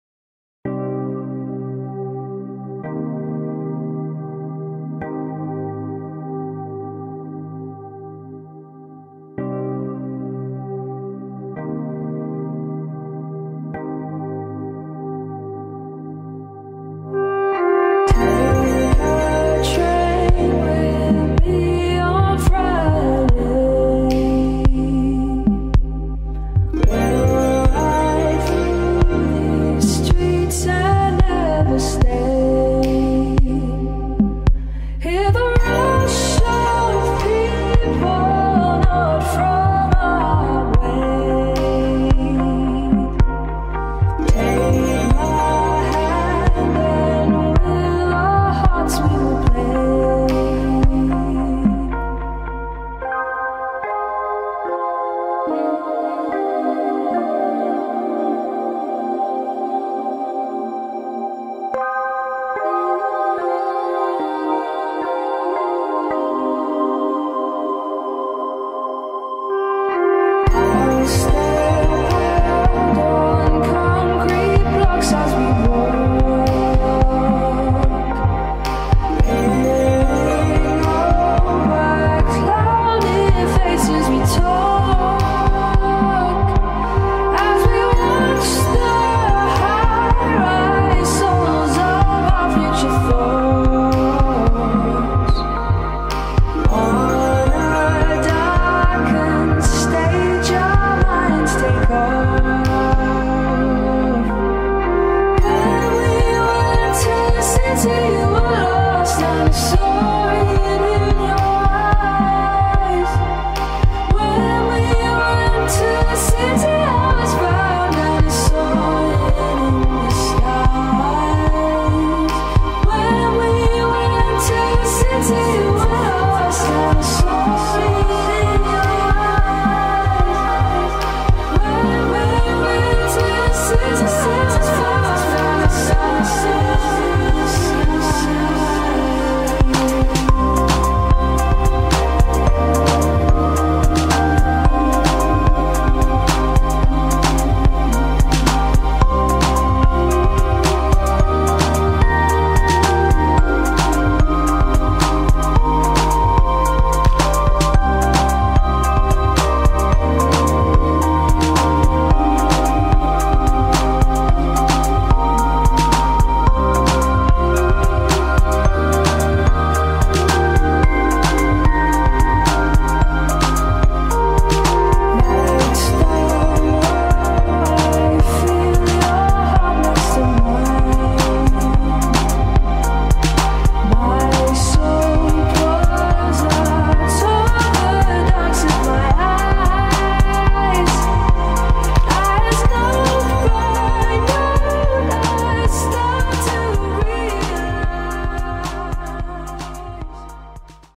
Chill Down (FPV Freestyle)
Just cruising and chillin down a little bit... no stress :) Flying FlightOne FalcoX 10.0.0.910! I hope you all like it!